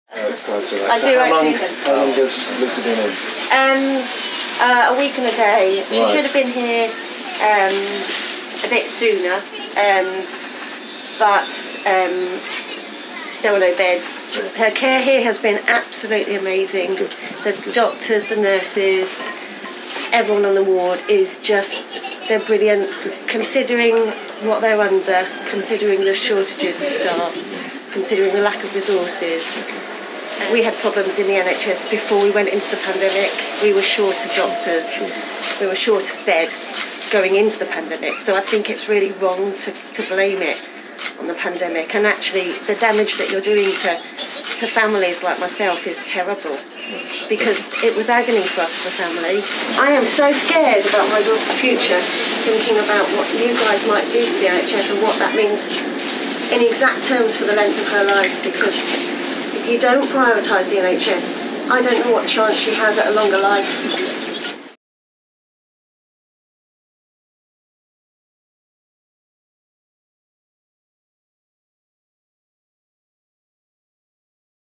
Mother of sick toddler grills Health Secretary over NHS conditions
‘The damage you’re doing to families is terrible’: mother challenges health secretary

A mother of a three-year-old girl suffering from cystic fibrosis has rebuked Steve Barclay over NHS staff working conditions as he visited a London hospital.

The Health Secretary was confronted by Sarah Pinnington-Auld when he met her and her daughter Lucy during a visit to King’s College University Hospital in Denmark Hill on Monday.

During a conversation by Lucy’s hospital bed, Ms Pinnington-Auld could be heard telling Mr Barclay that staff are “absolutely amazing” but pressures on the NHS are affecting the care for their daughter, whose recent bronchoscopy had to be delayed due to a lack of beds.